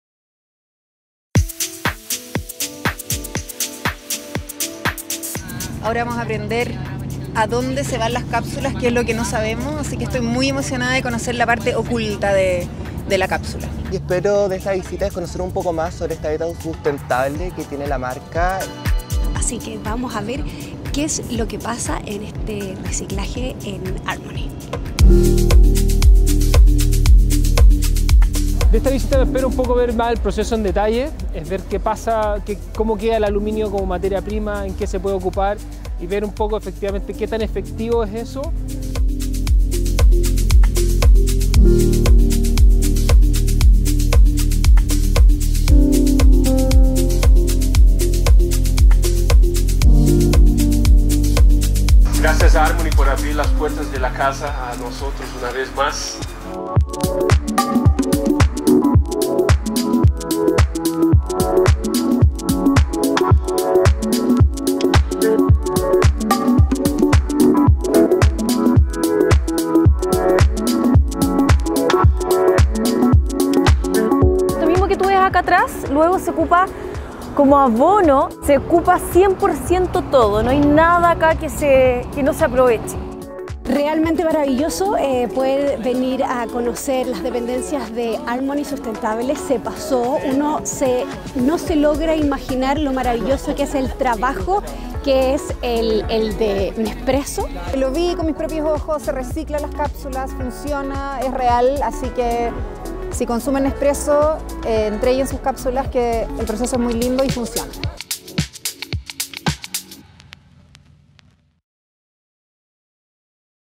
Nespresso - Descubre el ciclo de vida de las cápsulas Nespresso | CL
Junto a nuestros amigos de la marca visitamos la planta de Armony Sustentable, para mostrarles qué es lo que ocurre con nuestras cápsulas usadas luego de ser entregadas por nuestros clientes.